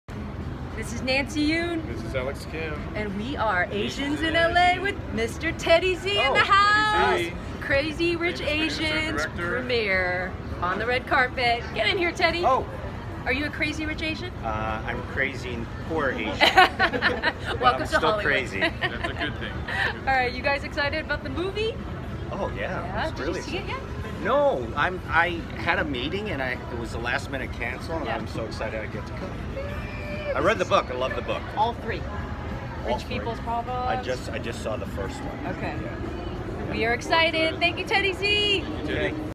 at Crazy Rich Asians premiere with legendary producer Teddy Zee!
On the red carpet with legendary Asian producer Teddy Zee, Producer! He produced movies over $2.6B in revenue with blockbuster hits like ThePursuitofHappyness with @willsmith Hitch CharliesAngels StarTrek VI.

Crazy Rich Asians